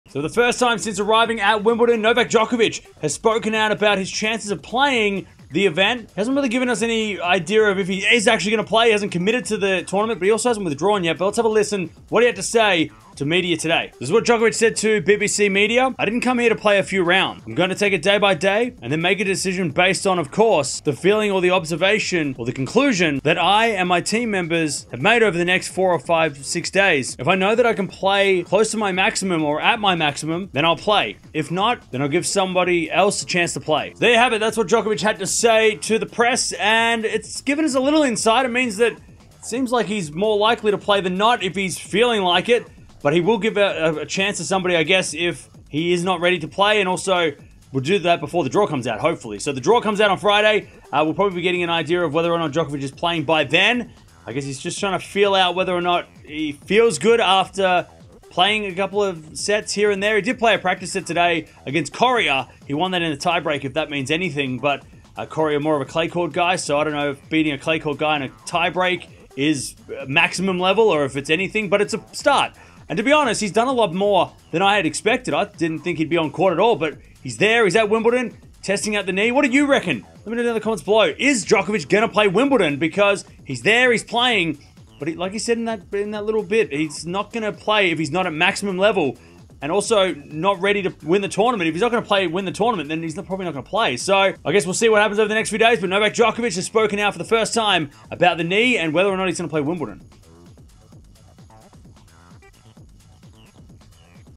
Djokovic Responds on Doubts of Playing Wimbledon 2024 | Tennis News
With Wimbledon 2024 just a week away, Novak Djokovic has spoken out to the media for the first time on whether here will play the event or not. He will wait over the next few days to see if he feels ready and only if he can give his maximum effort. Will Nole play?

0:00 | Intro
0:13 | Djokovic Interview
0:33 | Outro